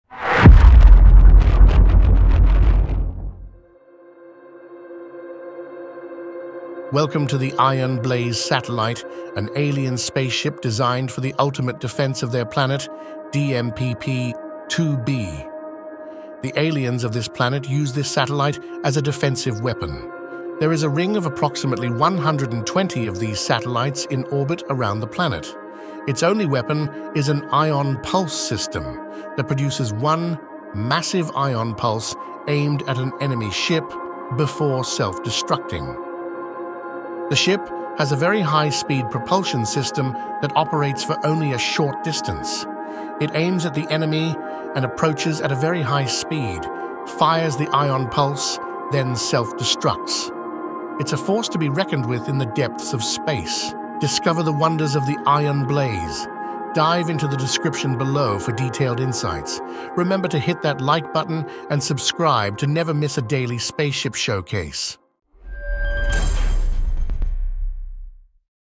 Daily Spaceship - Ion Blaze, Ion Pulse Weapon
Name: Ion Blaze
Planet of Origin: DMPP-2 b

Unveil the Ion Blaze, the ultimate alien weapon! Explore its advanced features and capabilities as it reigns supreme in the cosmos. 🚀